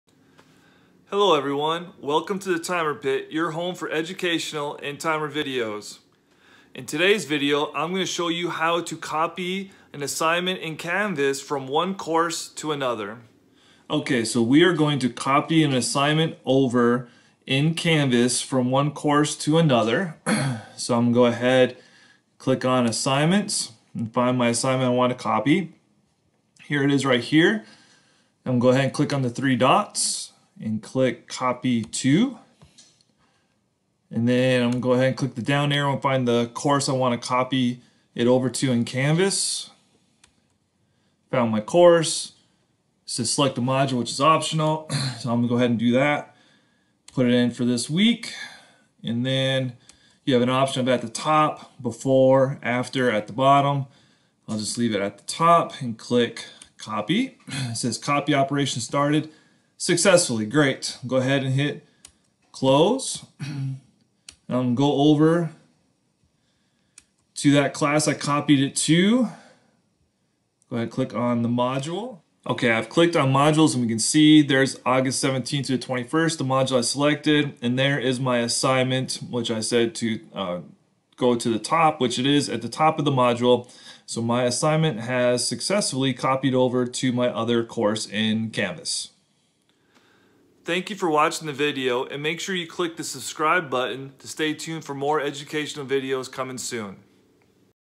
How To Copy An Assignment in Canvas From One Course & Move It To Another Course
This "How To" video goes over how to copy and assignment in canvas from one course and move it to another course! This will help you save a lot of time when you are planning for the week!